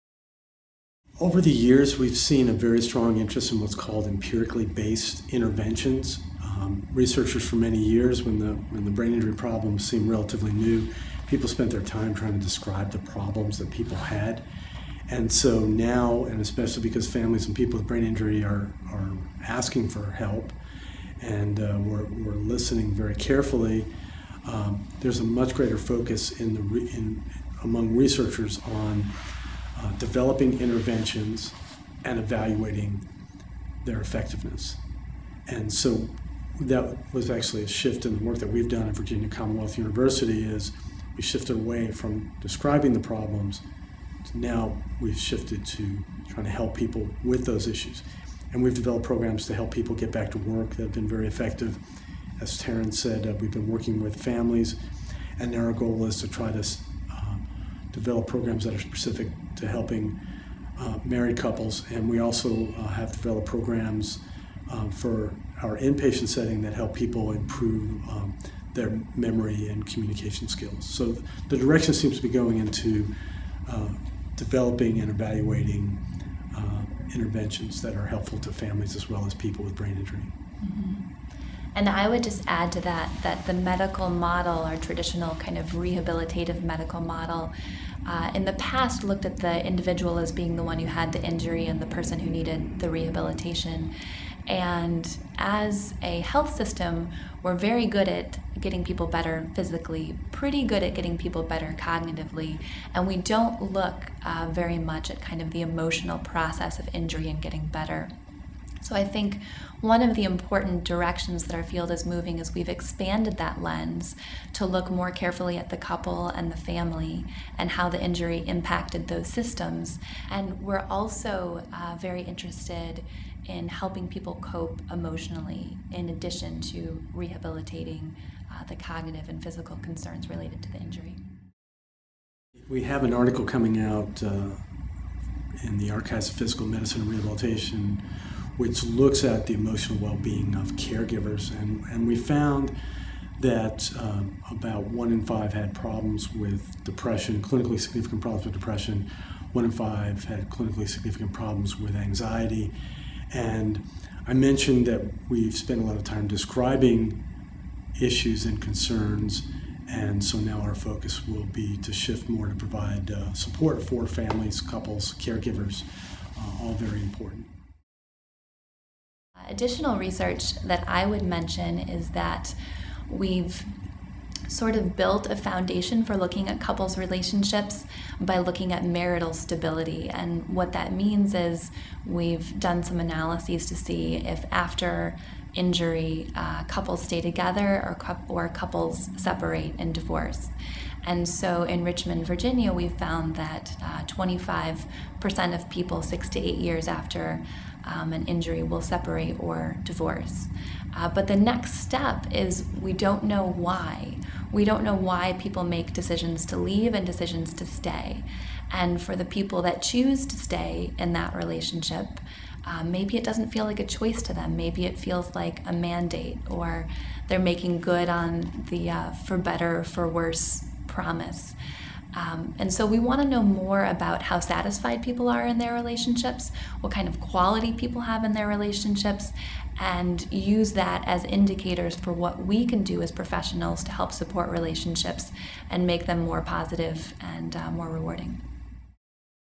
Research Areas and Interests
Jeffrey S. Kreutzer, PhD, ABPP, is the Rosa Schwarz Cifu Professor of Physical Medicine and Rehabilitation at Virginia Commonwealth University (VCU), Medical College of Virginia Campus. There, he is also a professor of Neurosurgery and Psychiatry. Dr. Kreutzer serves as Director of Virginia's federally designated Traumatic Brain Injury Model System and coordinates VCU Health System outpatient services for families and persons with brain injury. For the last two decades, he has been active in implementing empirically based vocational rehabilitation, psychological support, cognitive rehabilitation, and family support programs. Dr. Kreutzer has co-authored nearly 150 peer-reviewed publications, most in the area of traumatic brain injury and rehabilitation. Co-Editor-in-Chief of the international journals Brain Injury and Neurorehabilitation, he has also published a dozen books focused on topics including vocational rehabilitation, community integration, behavior management, and cognitive rehabilitation. Currently, he serves as Editor-in-Chief of the soon to be published by Springer, New York, Encyclopedia of Clinical Neuropsychology.

Taryn M. Stejskal, PhD is a licensed marriage and family therapist and an Advanced Rehabilitation Research Training (ARRT) postdoctoral research fellow at Virginia Commonwealth Medical University (VCU) in the division of neuropsychology in the department of Physical Medicine and Rehabilitation (PM& R). She received her masters degree in Marriage and Family Therapy (MFT) as well as her doctoral degree from the University of Maryland, College Park (UMCP). At present, Dr. Stejskal is the family support and education coordinator at VCU. She conducts the Brain Injury Family Intervention (BIFI), funded by National Institute of Disabilities and Rehabilitation Research (NIDRR), a clinical research program designed to help families and couples gain necessary education, psychological support, and personal skills after brain injury. Dr. Stejskal trains clinicians to implement the BIFI and presents her work nationally as well as internationally. She has written numerous peer reviewed research and newsletter articles on the impact of brain injury on couple and family relationships. Dr. Stejskal is also a volunteer co-facilitator for the Brain Injury Association of Virginias (BIAV) family support group, and she is an executive board member of the Community Futures Foundation (CFF).

On April 24th, 2009, BrainLine had the opportunity to sit down with Dr. Kreutzer and Dr. Stejskal to talk about their work with traumatic brain injury survivors and their families.